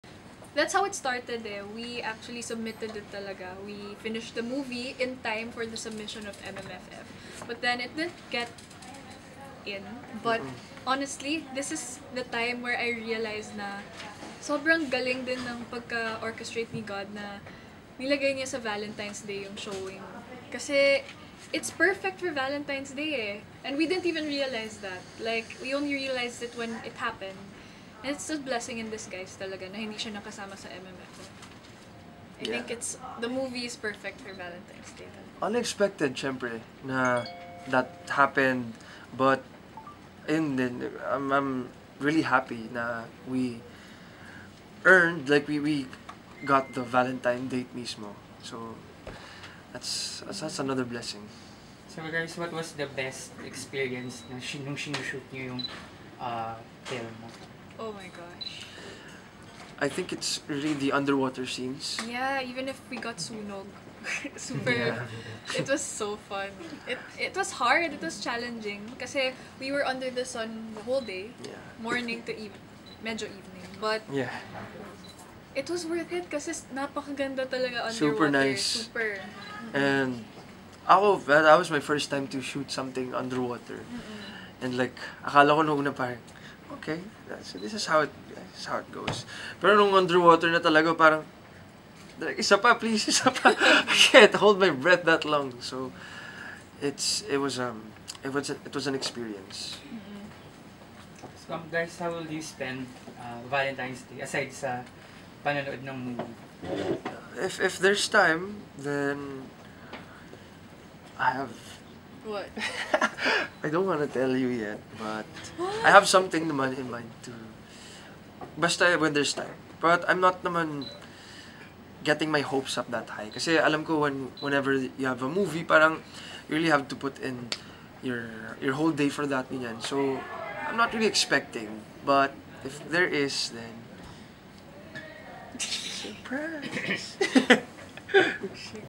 ElNella talks about My Fairytale Love Story (Tunog Starmagic 2018)
Bakit nga ba hindi napasama sa MMFF Yung My Fairytale Love Story? At ano nga ba plano ng ElNella this Valentine`s Day?

Support their upcoming movie this coming Valentine`s Day (February 14 2018)